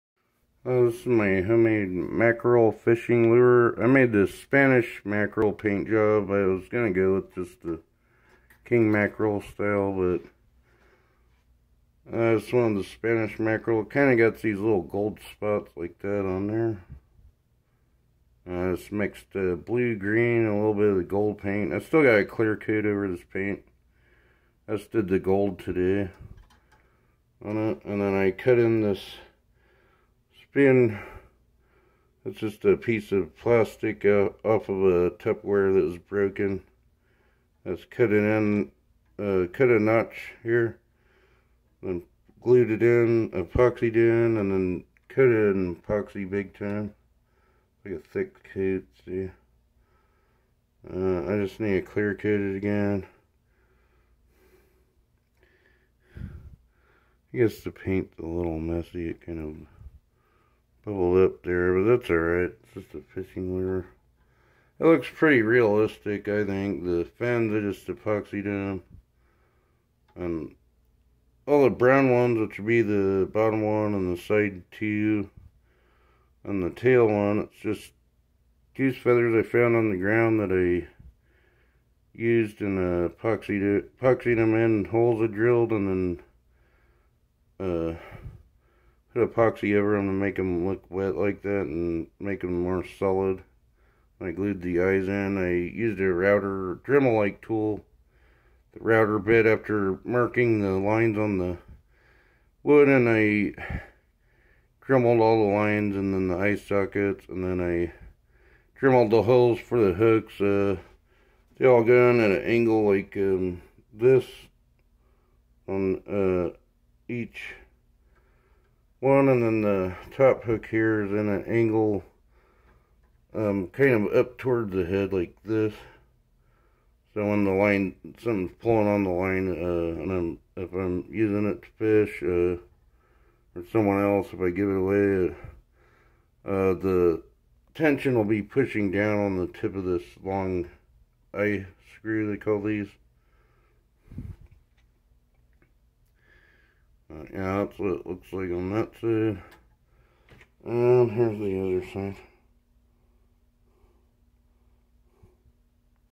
mackerel fishing lure part 6
I decided to go with Spanish mackerel look and paint job.